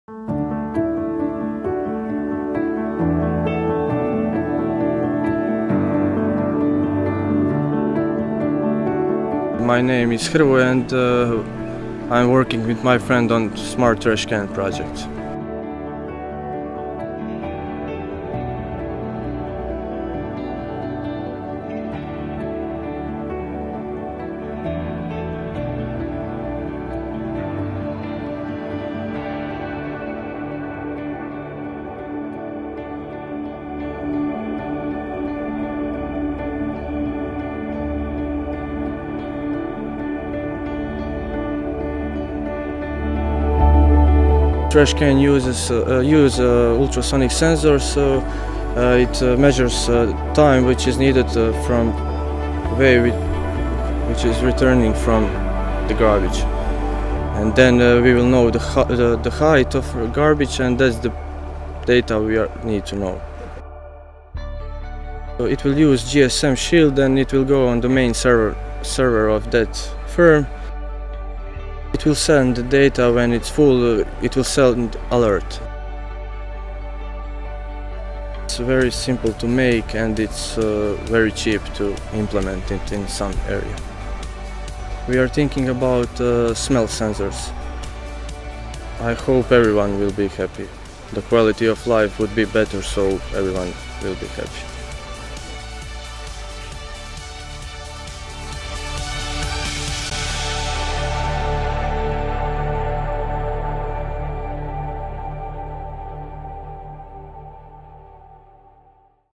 CityOS Hackathon Dubrovnik: SMART TRASH CAN
Smart trash can is an eco friendly product based on the Arduino Uno board. This trashcan is able to measure the height and weight of garbage stored in it. This is achieved with ultrasonic sensor, transmitted wave returns back on ultrasonic receiver. Knowing the speed of sound and using the formulas of motion we know the distance between sensor (top of the can) and garbage. This data is converted to percentage of used space. Second ultrasonic sensor is situated on the bottom cap which is mounted on springs. Knowing the specifications of springs and using the basic physics laws we know mass of garbage. All of data is sent to main server which can be used by communal firms that can collect that garbage when is necessary. Also, collected data can define useful statistics that can be a basis for futher steps. Optimized energy consumption and better quality of life are the main goals of this project.